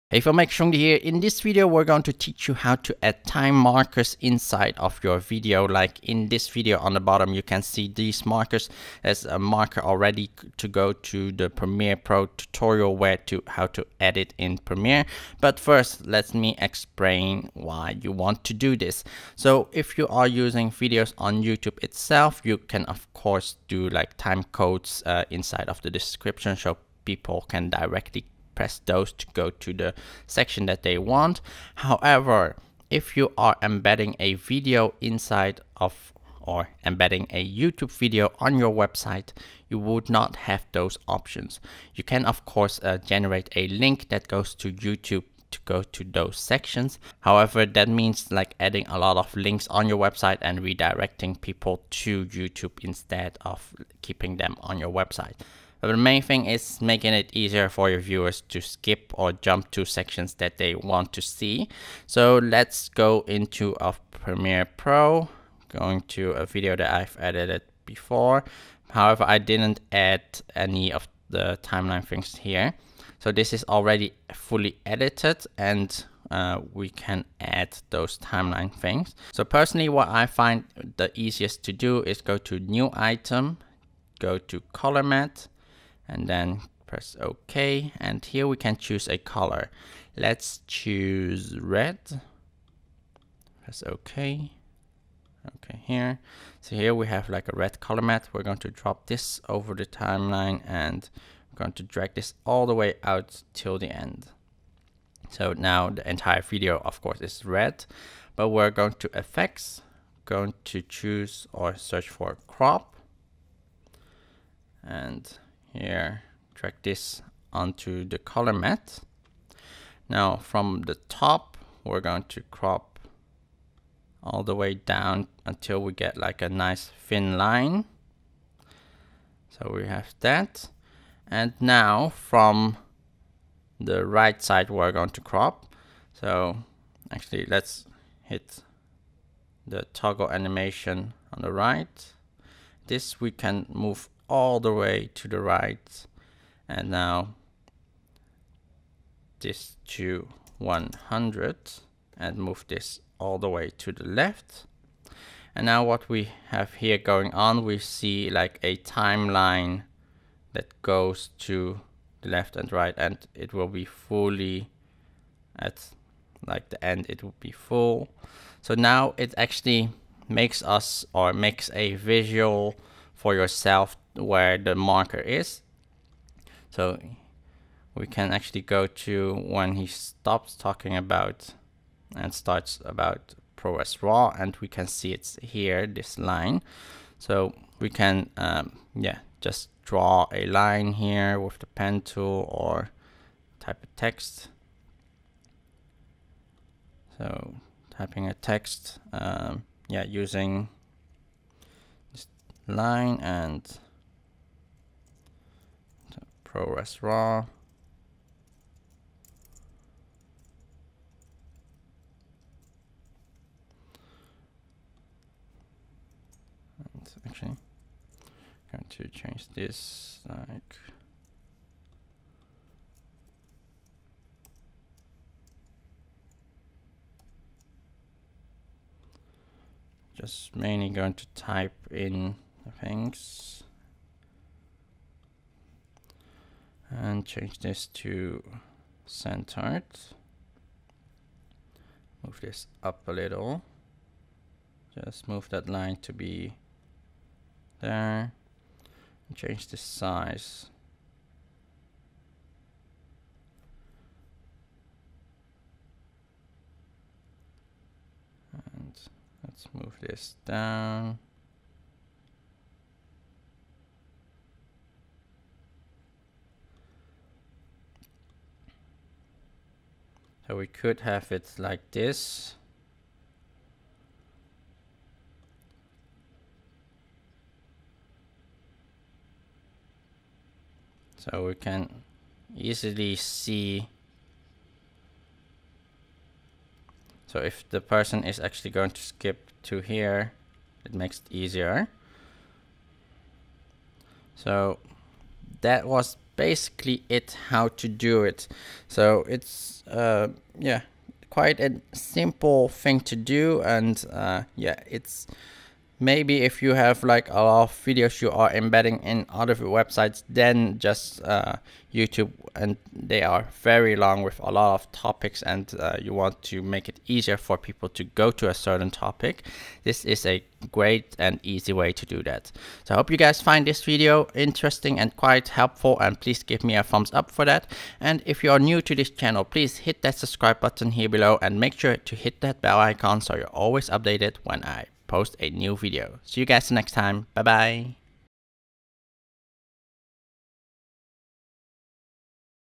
Embedded time markers with Adobe Premiere Pro Chung Dha
In this premierepro videoediting tutorial I am showing you how to add embedded time markers in your Youtube videos so it's easy to show viewers which sections to go to quickly without needing to go into the description. Or when you embed your videos on other website it will still be easier to jump to the sections they want to see.